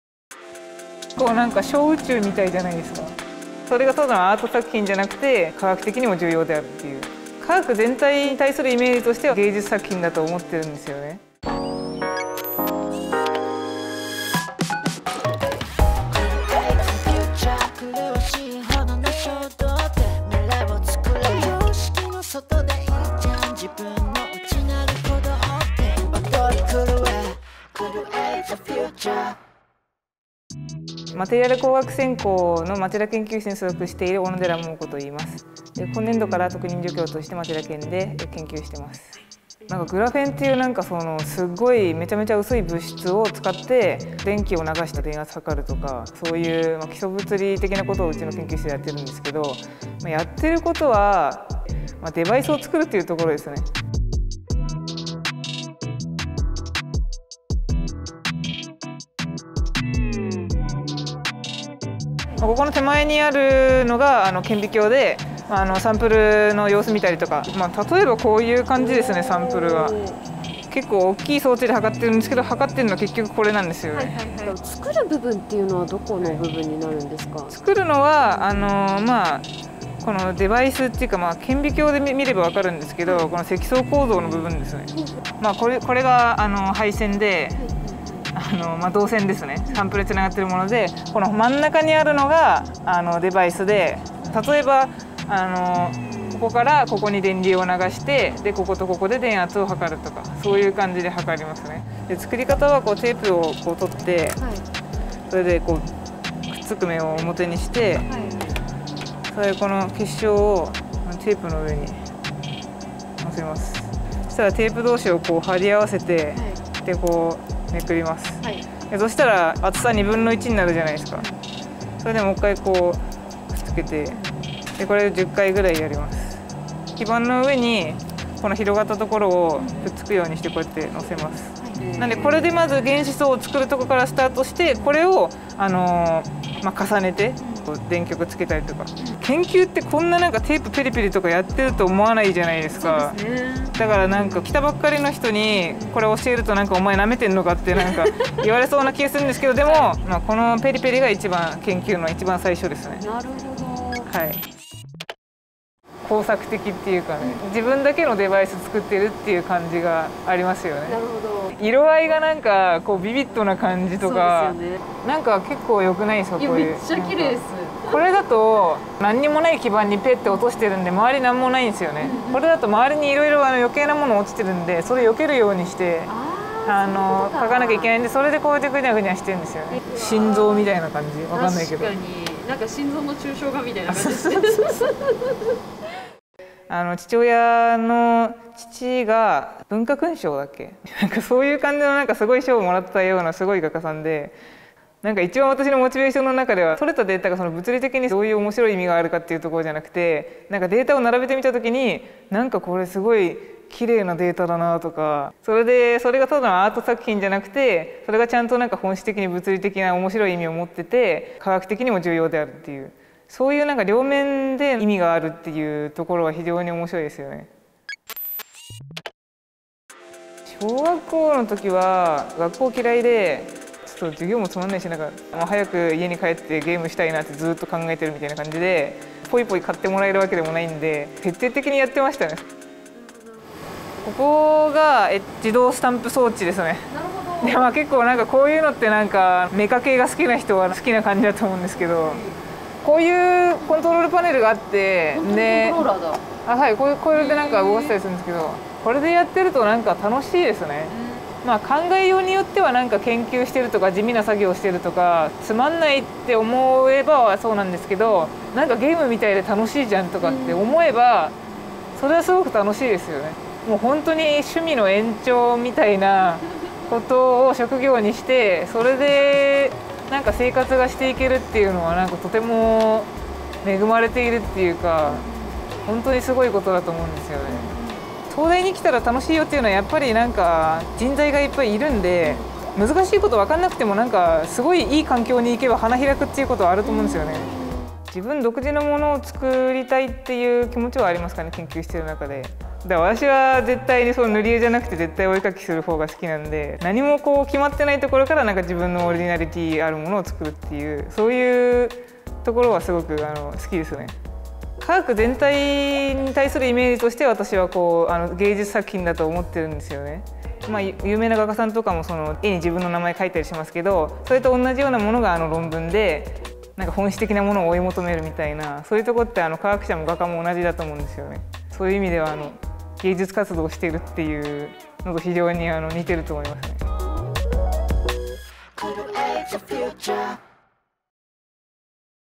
【東京大学 UTokyo】科学で芸術作品を創りたい。I want to create a work of art with science.(JP/EN)
This Youtube video has subtitles.

小野寺 桃子　Momoko Onodera
東京大学大学院工学系研究科マテリアル工学専攻
町田研究室 博士課程修了
　　　　　　　　　　　　　　　　　　　　　　　　　　　　　　　　　　　　　　　　　　　有名な日本画家を祖父に持つ小野寺さんは、グラフェンという物質で新しいデバイスを作るための研究をしてる。

グラフェン研究は、黒鉛を薄くするためにセロハンテープで結晶をぺりぺりめくるところからスタートし、その工程はまるで工作やゲームのようだと彼女は語る。

町田研に揃っている様々な装置を使って、職人のように作られたデバイスは美しく、まるで小宇宙のよう。

科学全体が芸術作品であると言う彼女が見る、デバイスの小宇宙とは！？

2021年4月撮影、所属等は当時

Momoko Onodera, whose grandfather is a famous Japanese-style painter, is conducting research on the creation of new devices with graphene. Graphene research was begun by flipping crystals with cellophane tape to thin the graphite. She says the process is like a craft or game. A device made like a craftsman is beautiful, like a microcosm. Her regard of a device as a microcosm suggests that science as a whole is a work of art!